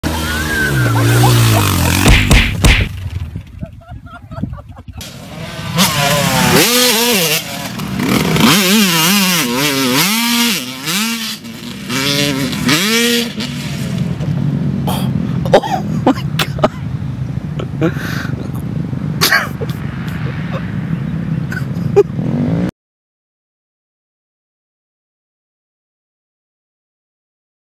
Motocross Fails: Jordon Smith goon riding crash - Motocross Action Magazine
For this Motocross Fails video we found this old video of Jordon Smith goon riding out at MTF in Georgia. Some times things go wrong even when your playing around!